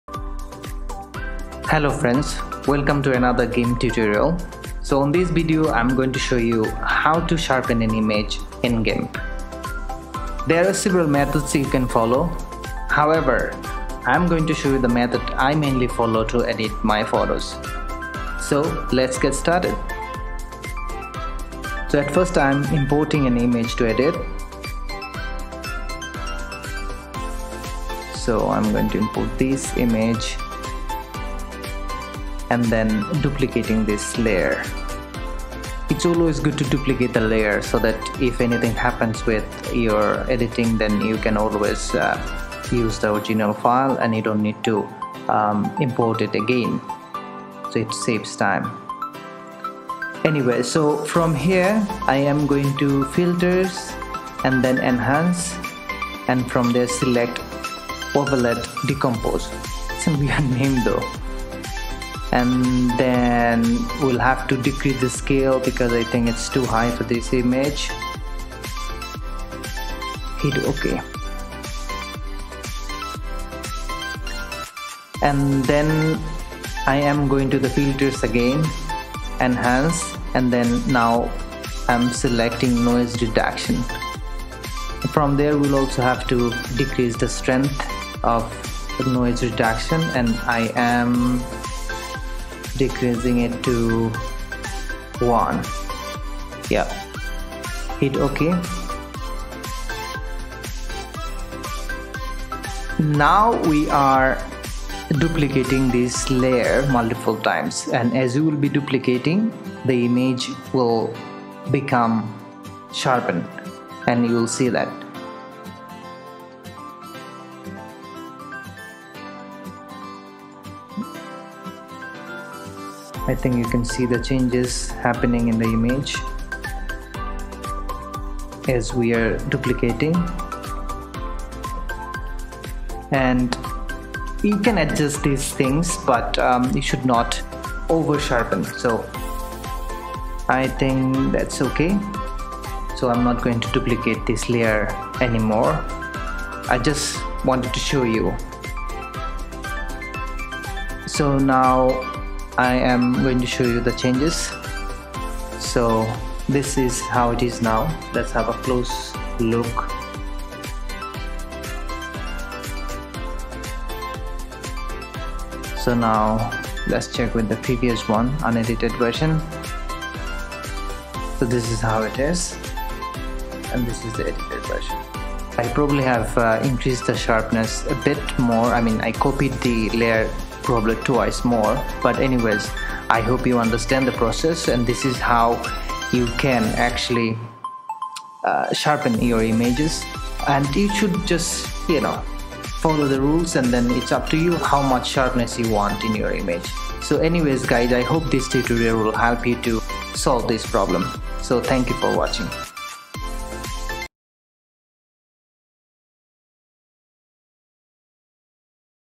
How to Sharpen Images in Gimp 2.10.22 | Learn Using Wavelet Decompose Plugin to Sharpen Photos
In this video, we are going to demonstrate the process of sharpening images in the Gimp. There are several techniques available to sharpen images in the Gimp. However we are going to follow a simple method  to sharpen an image.

Thanks,
LI